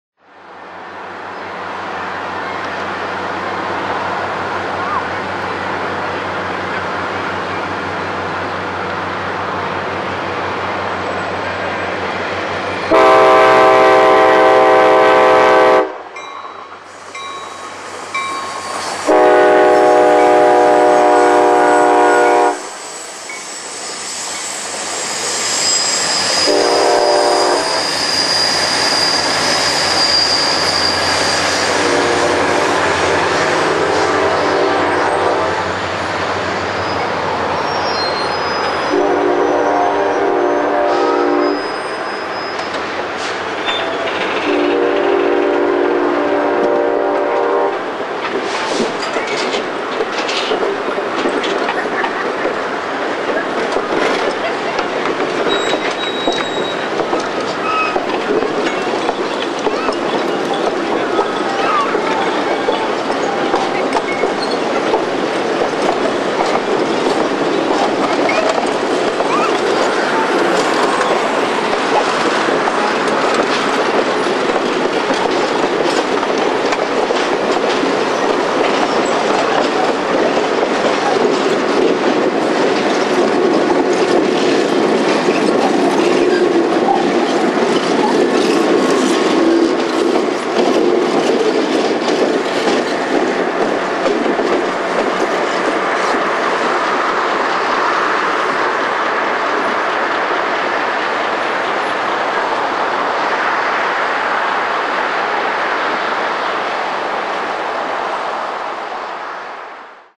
ACE #6 & Niles Canyon Railway Train Of Lights At Sunol
Here is the afternoon/evening Altamont Commuter Express or ACE #6 making its way out of Niles Canyon through the tiny town of Sunol, CA while the Niles Canyon Railway Train of Lights heads back to Niles.  Enjoy!!  Shot 12/14/12